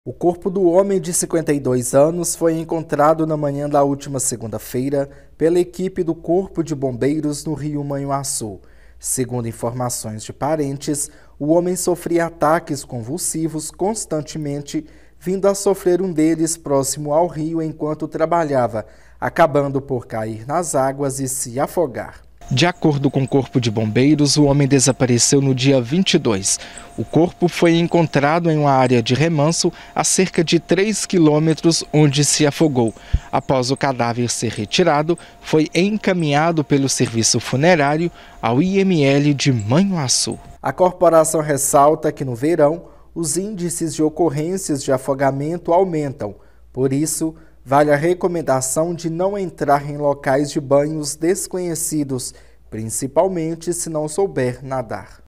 BOMBEIROS ENCONTRAM CORPO DE HOMEM DESAPARECIDO APÓS SE AFOGAR NO RIO MANHUAÇU
O corpo do homem, de 52 anos, foi encontrado na manhã da última segunda-feira (24) pela equipe do Corpo de Bombeiros.

Segundo informações de parentes, o homem sofria ataques convulsivos constantemente, vindo a sofrer um deles próximo ao rio enquanto estava trabalhando, acabando por cair nas águas e se afogar.

De acordo com os bombeiros, o corpo foi localizado em um área de remanso a cerca de 3 km do local onde o homem submergiu. O cadáver foi retirado e encaminhado ao IML de Manhuaçu.

A corporação ressalta que, no verão, os índices de ocorrências de afogamento aumentam. Por isso, vale a recomendação de não entrar em locais de banho desconhecidos, principalmente, se não souber nadar.